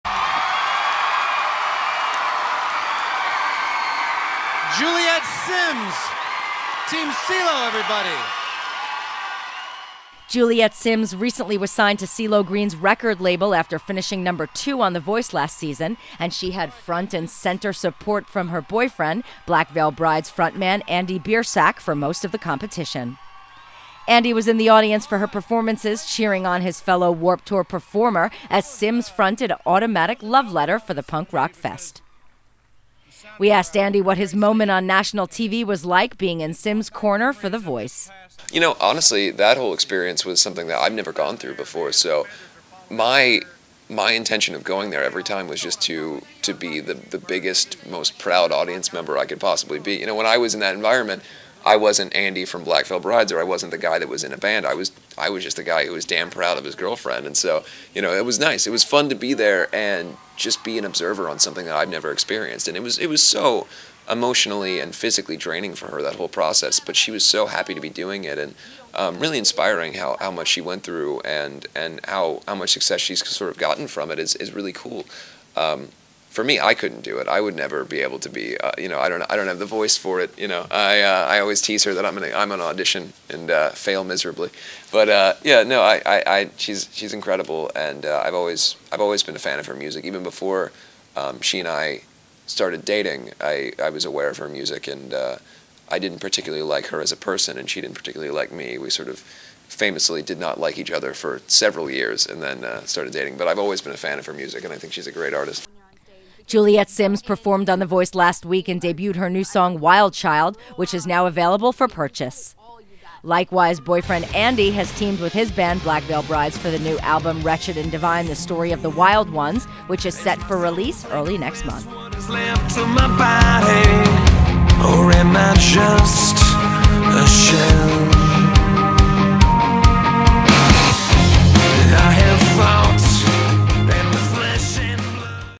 BLACK VEIL BRIDES ANDY RECALLS THE VOICE, GIRLFRIEND JULIET SIMMS STRONG SHOWING
Black Veil Brides Andy Biersack recalls his moment on The Voice vicariously through girlfriend Juliet Simms during last season.